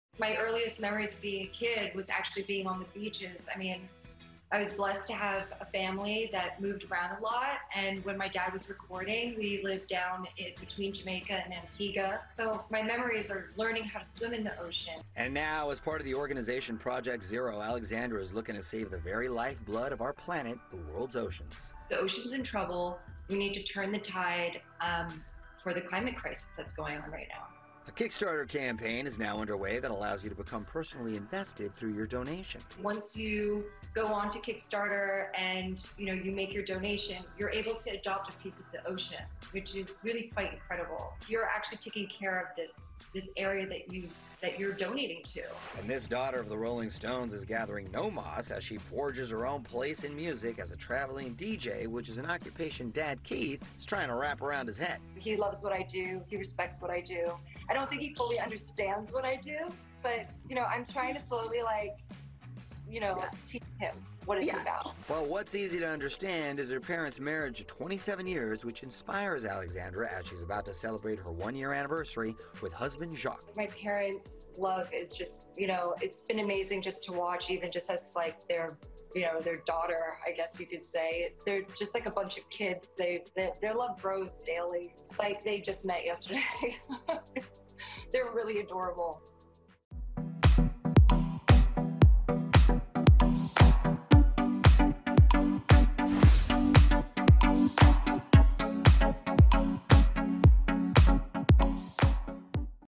Keith Richard's Daughter On Her Parents' 'Adorable' Love
Alexandra Richards is the daughter of rock star Keith Richards and supermodel Patti Hansen. She's now dedicating her life to combating climate change with a new initiative called Project Zero. Alexandra told Access Hollywood about her new project, her career as a DJ and what it was like growing up in the spotlight. She also dished on her parents' enduring love, saying, "They're just like a bunch of kids. Their love grows daily, like they just met yesterday. They're really adorable."